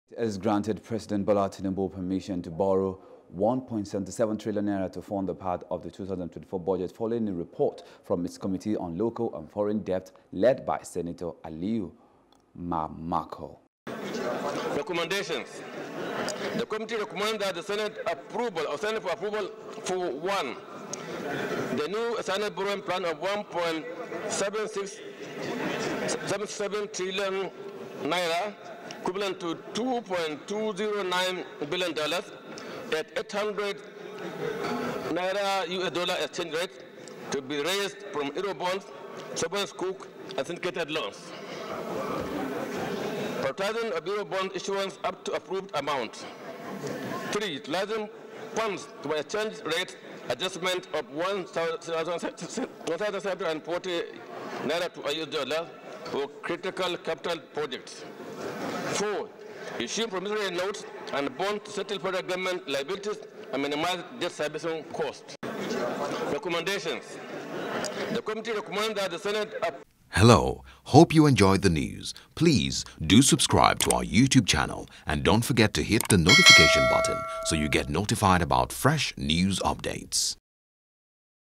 2024 Budget Senate Approves Tinubu’s Fresh $2 2billion Loan Request
The Nigerian Senate has granted President Bola Tinubu permission to borrow N1.77 trillion to fund part of the 2024 budget, following a report from its committee on Local and Foreign Debt, led by Senator Aliyu Wammako.

Deputy President of the Senate, Jibrin Barau, stated that the approval of a new external borrowing of N1.77 trillion was in the country's interest.

President Tinubu had earlier written to the Senate to approve N1.77 trillion to fund the 2024 Appropriation Act deficit..